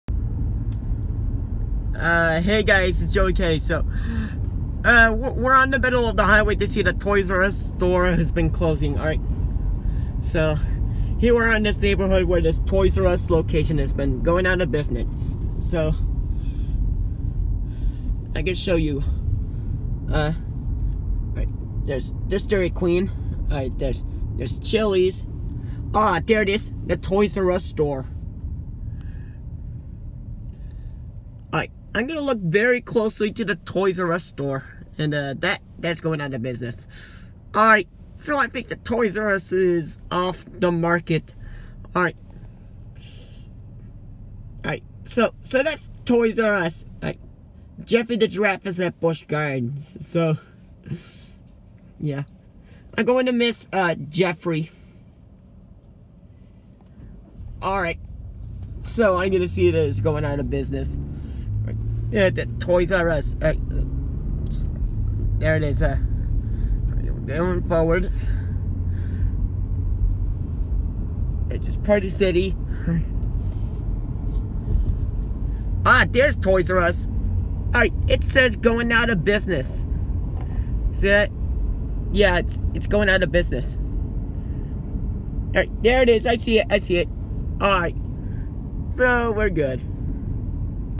Toys R Us location closing.
We are at the Toys R Us location and it’s going out of business.